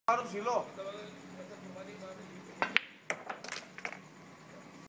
Snooker Tricks Karachi
Some tricks of snooker by Babar.